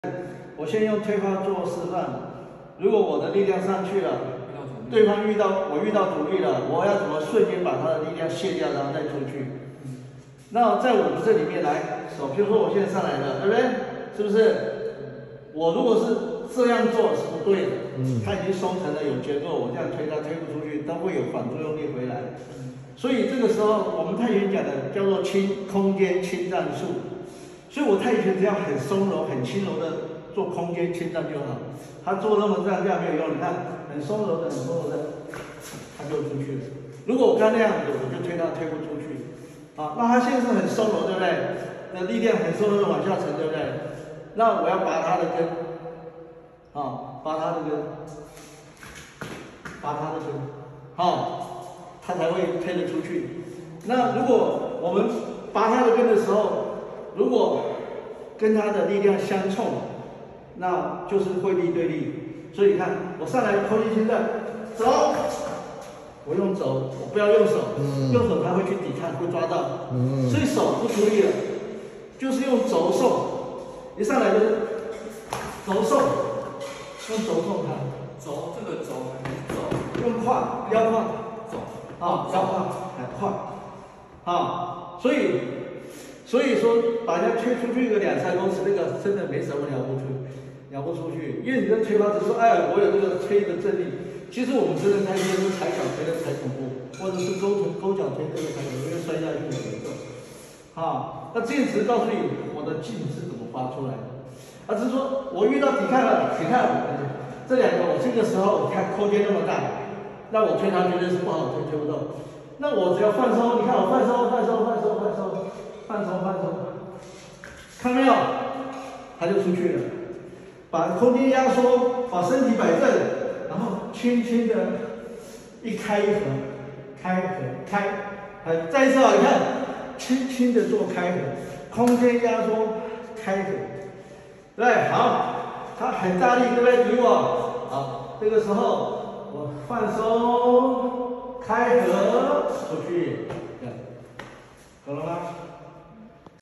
太極拳摔打拿踢之實戰用法太極推發時遇阻力如何化解
楊式太極真正的實戰形態推發時遇阻力時要如何化解